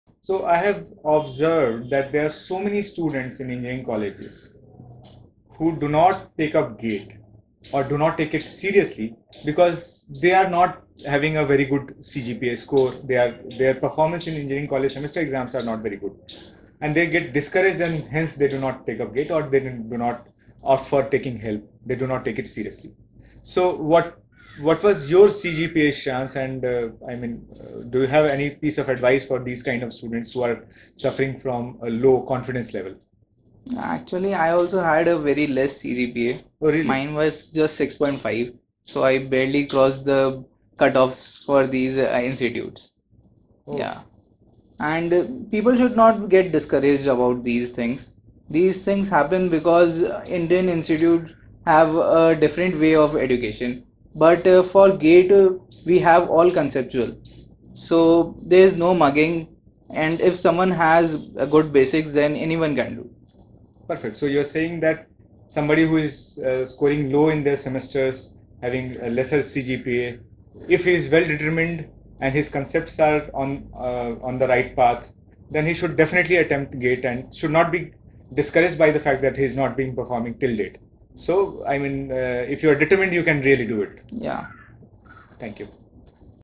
GATE AIR 9- Low CGPA, Don't Be Afraid | GATE Exam
Tips and experience of GATE AIR 9 Sreyans Nahata who done his B.tech in ECE and chose CS for GATE exam still managed to get GATE AIR 9. He faced few challenges but THE GATE ACADEMY video lectures helped him a lot.

Correspondence Course- Comprises of hard-copy GATE focused books options of Tests and video lectures. Its popular amongst self-preparing GATE aspirants.